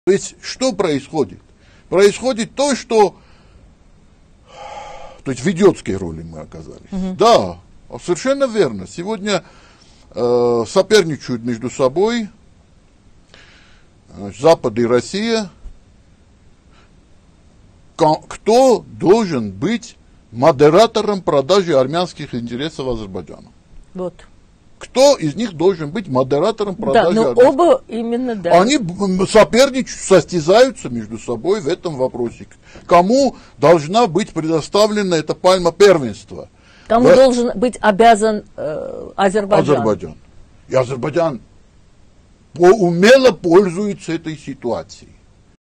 Амаяк Ованнисян: Запад и Россия конкурируют между собой, кто продаст интересы Армении Азербайджану.
Президент ассоциации политологов Армении Амаяк Ованнисян: «Мы оказались в идиотской ситуации. Запад и Россия конкурируют между собой, кто продаст интересы Армении Азербайджану. И Азербайджан умело пользуется этой ситуацией».